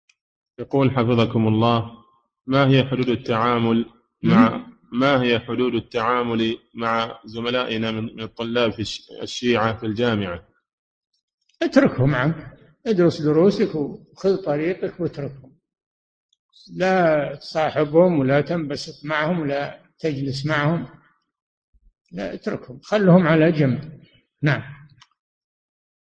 [353 -521] ما هي حدود التعامل مع زملائنا من طلاب الشيعة في الجامعة؟ - الشيخ صالح الفوزان
-[353 -521] السؤال: ما هي حدود التعامل مع زملائنا من طلاب الشيعة في الجامعة؟
الجواب: اتركهم عنك ادرس دروسك وخُذ طريقك واتركهم، لا تُصاحبهم ولا تنبسط معهم، ولا تجلس معهم، اتركهم خلهم على جنب.
للاشتراك في قناة الشيخ صالح الفوزان

للاستفسار والإقتراحات والدعم
رئيس الفريق
لمشروع كبار العلماء